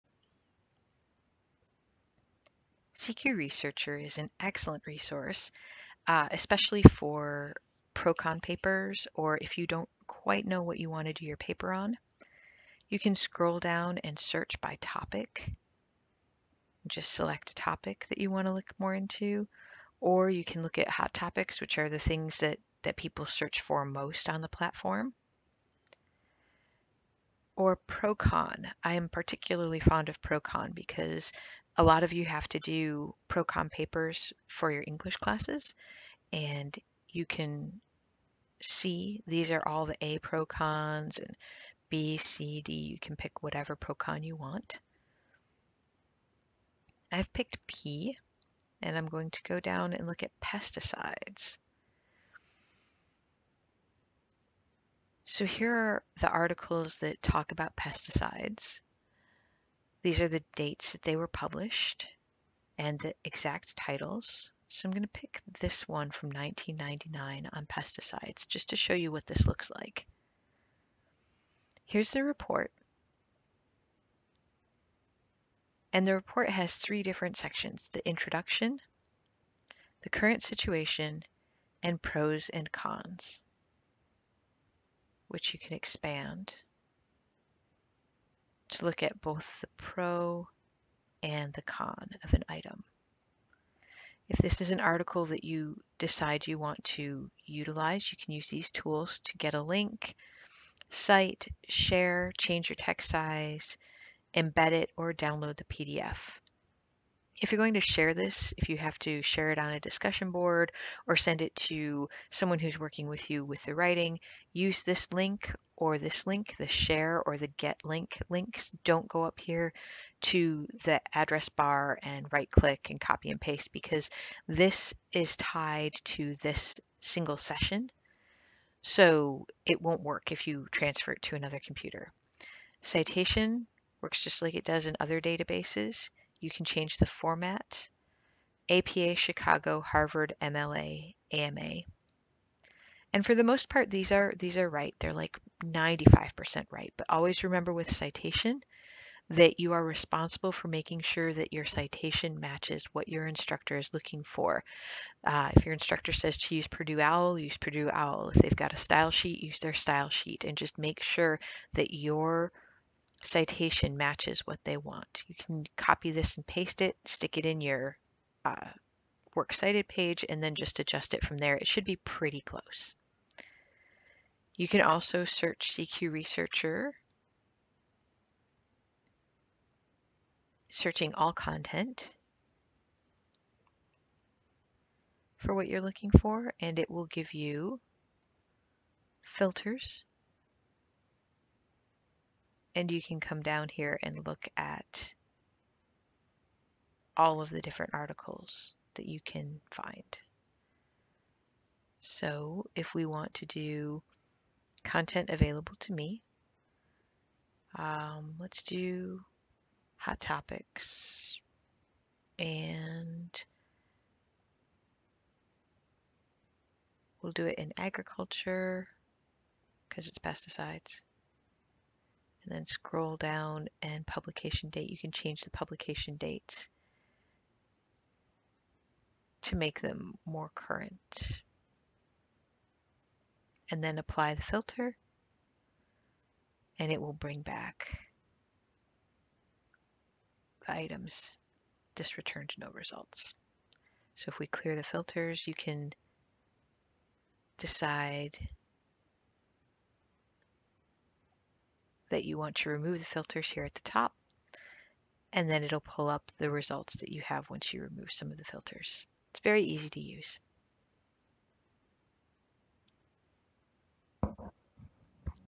Using CQ Researcher
How to search CQ Researcher and cite the findings.